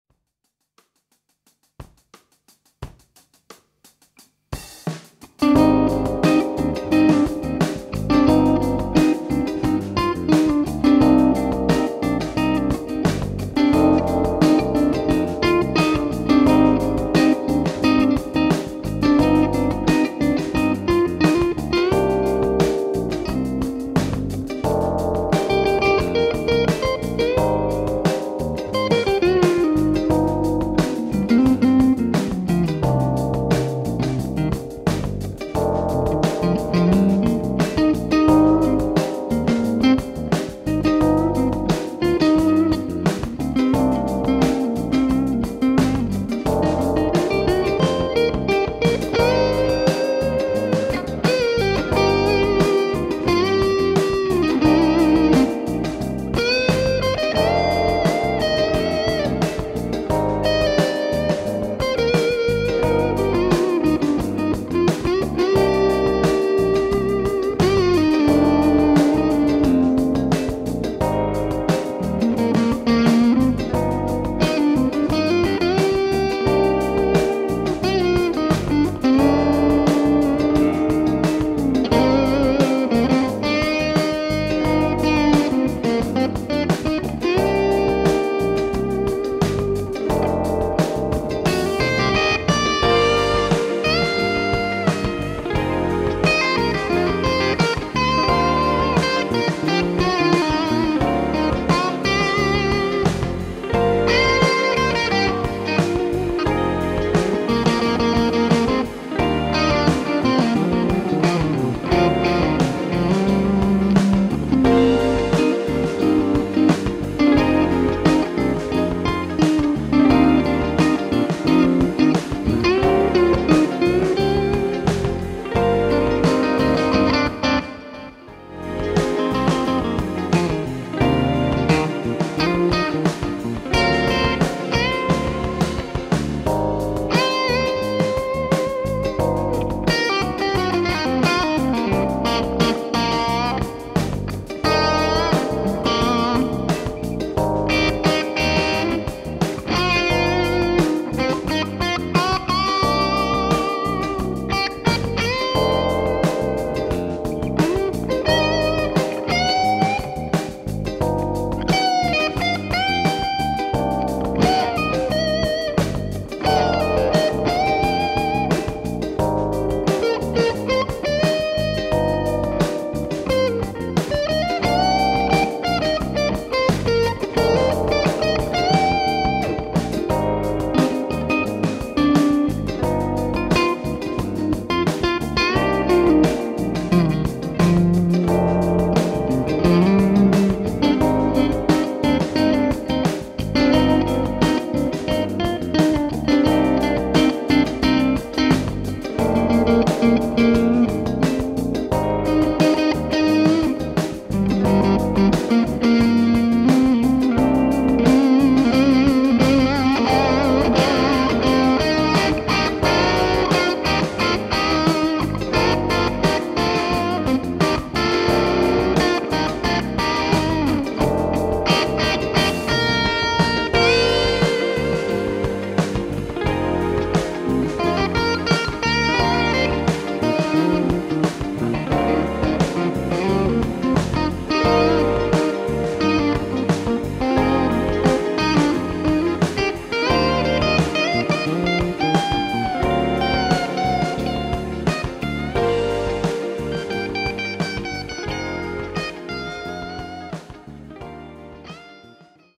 ++ Faber Revival Model STC (2TS) - GUITAR DEMO ++
Faber STC (#4)
guitarist: Alex Schurr

Eine tolle E-Gitarre zum schmalen Preis, die sich absolut sehen und hören lassen kann.
Mit einem Wilkinson Humbucker an der Bridge, und je 1 Single Coil Pickup in der Mitte und am Neck,
verchromte Hardware, Vintage Style Tremolo bei der Farbe vintage white.
Bei den anderen Farben sind 3 Wilkinson Single Coil Pickups verbaut.
Body: Erle
Hals: Ahorn, einteilig
Griffbrett: Ahorn bei der Farbe Black, bei allen anderen Farben Palisander.
Verfügbare Farben: black (BB), vintage white (VW), signalrot (RD), 2-Tone Burst (2TB)

backing track: Sebastien Zunino

Recorded with Focusrite 2i2, Carr Viceroy Amp (Fender Deluxe Type), Zendrive, OCD (for S-Style Guitars), TC electronics Delay and Reverb, Hall of Fame